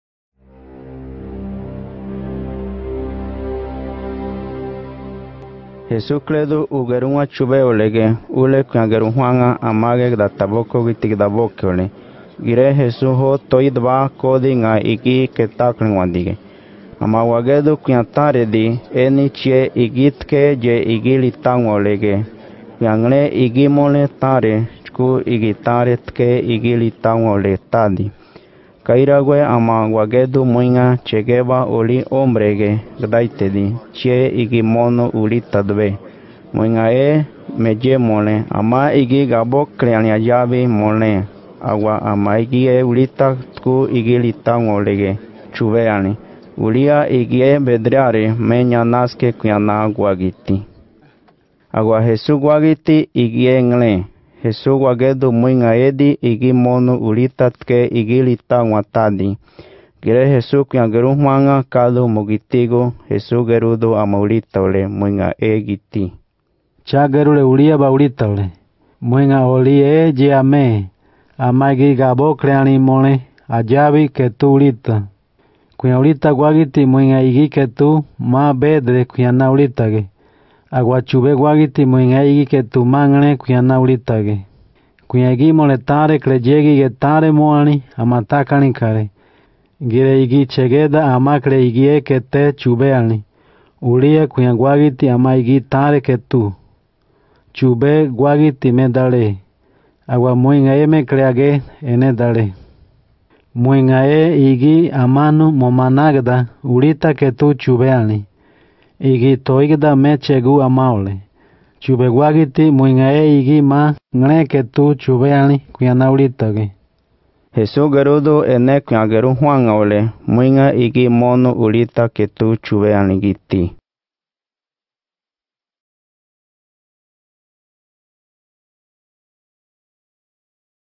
Buglere - Marcos 12.41-44 “La ofrenda de la viuda” [sab]
Videos de las Escrituras en la lengua de Buglere de Panamá. [sab]
Visita ScriptureEarth para más materiales.